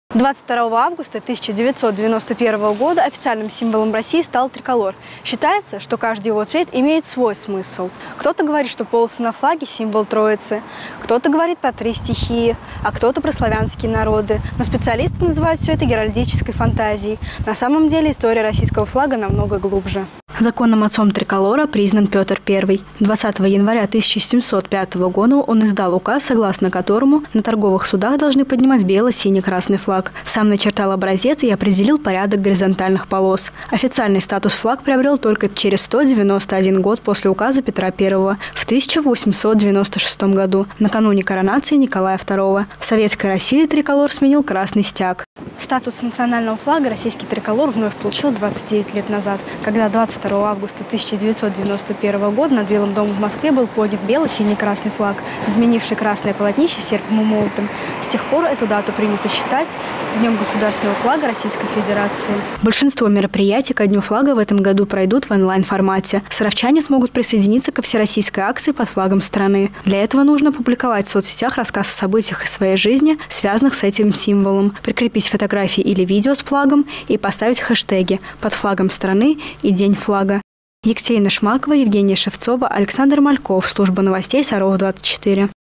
День Государственного флага России
22 августа в России отмечают День Государственного флага. Российский триколор имеет более чем 300-летнюю историю. Впервые бело-сине-красный флаг с нашитым на нем двуглавым орлом был поднят на первом русском военном корабле "Орел".